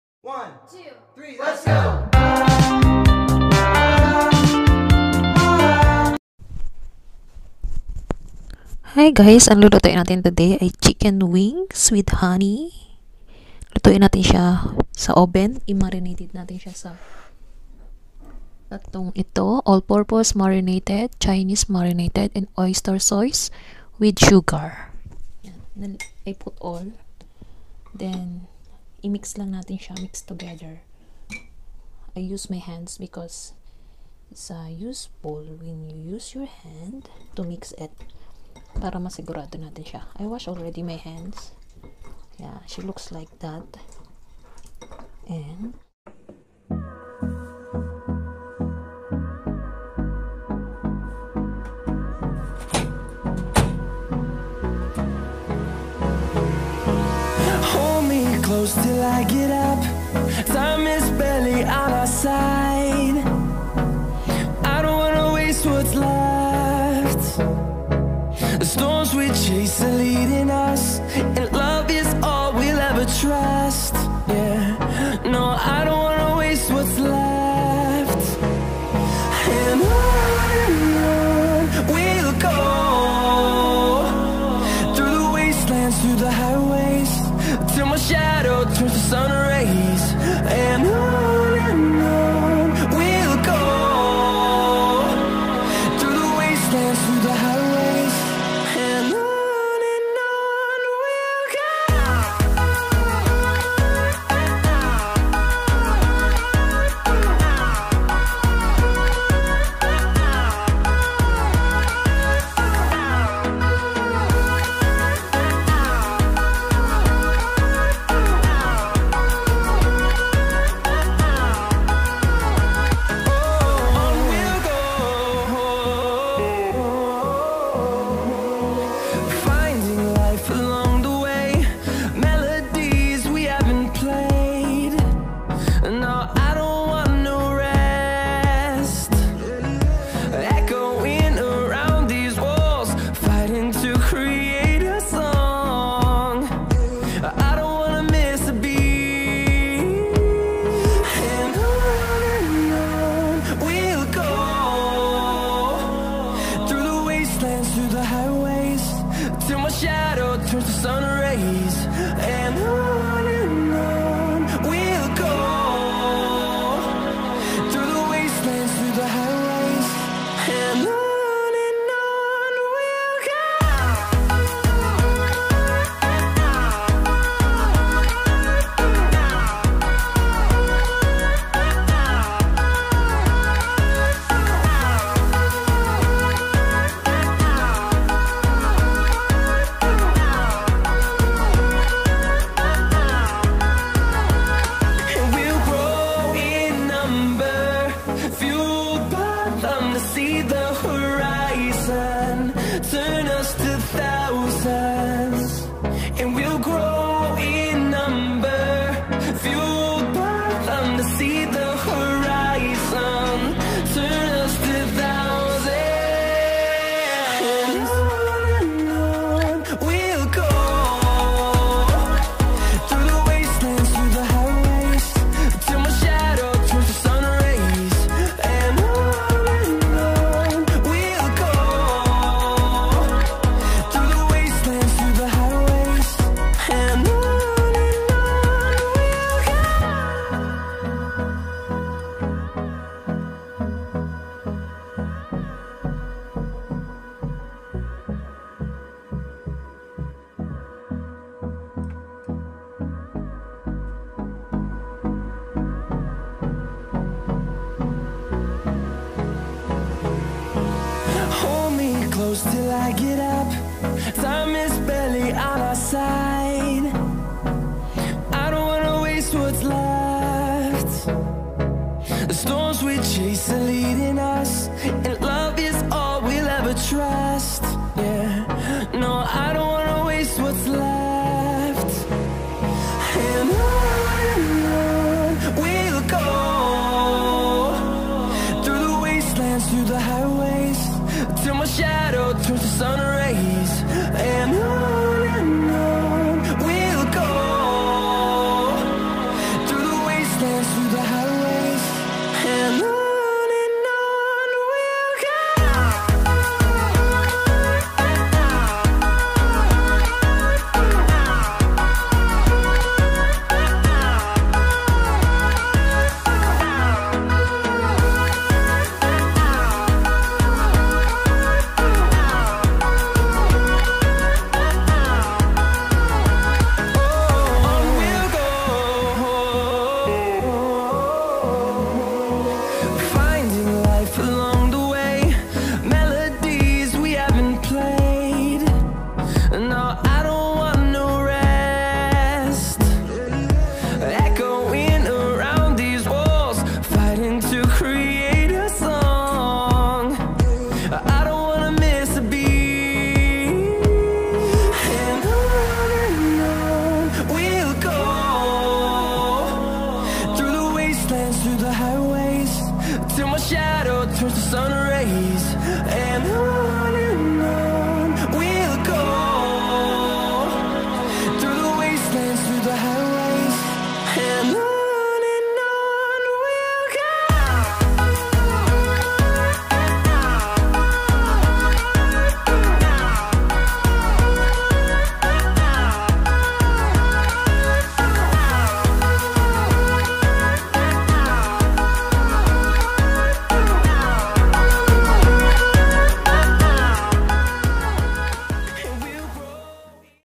chinese recipe_chiken wings with barbeque honey sauce;) it's a yummy try to cooking and taste it;)
chinese recipe#hongkong life#cooking time
hope you try to cook